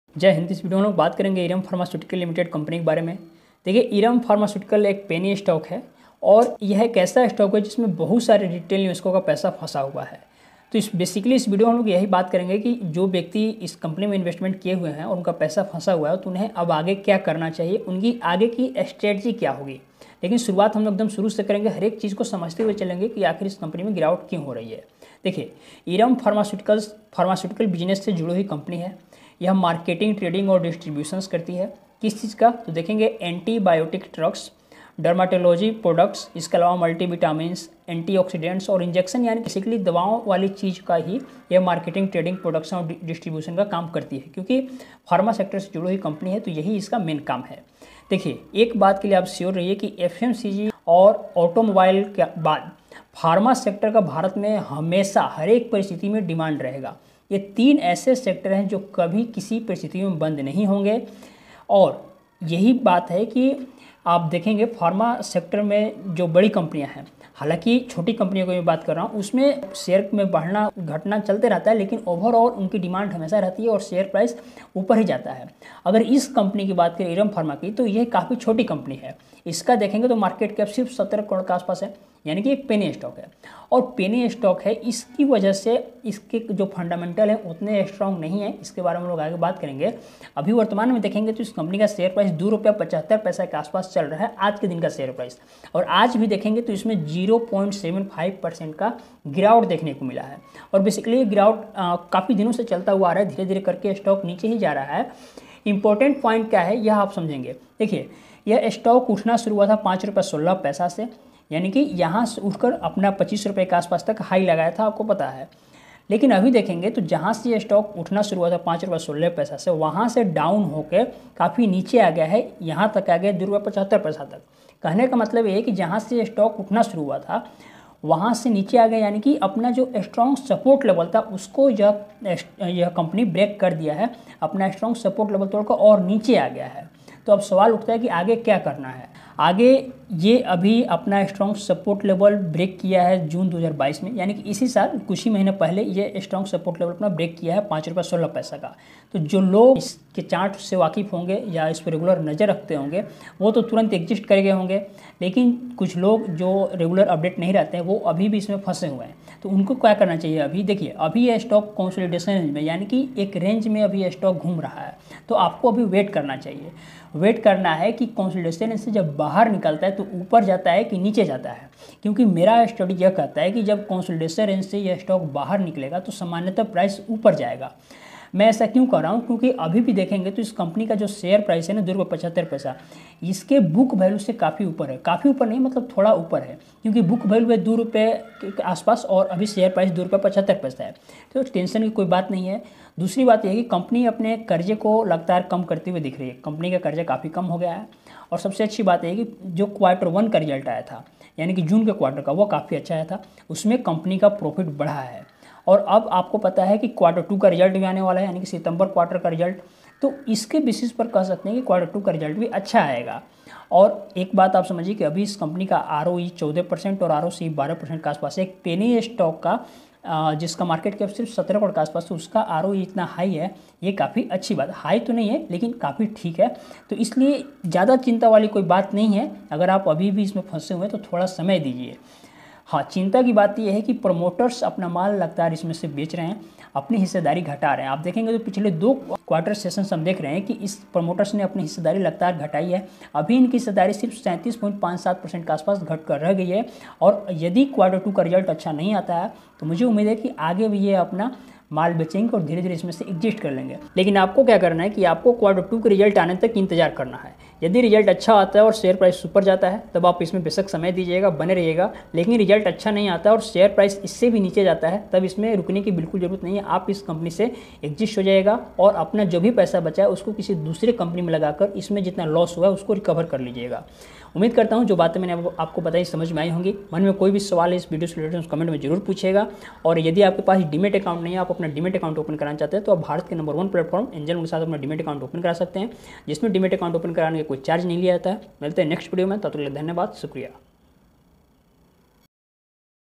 Earum pharmaceuticals ltd share price | Earum share latest news today | Future of india
In this video you will know about Earum pharmaceutical share latest news and updates .

Open Free Demat Account in Angel One -

Hash Tags -

Please Note:-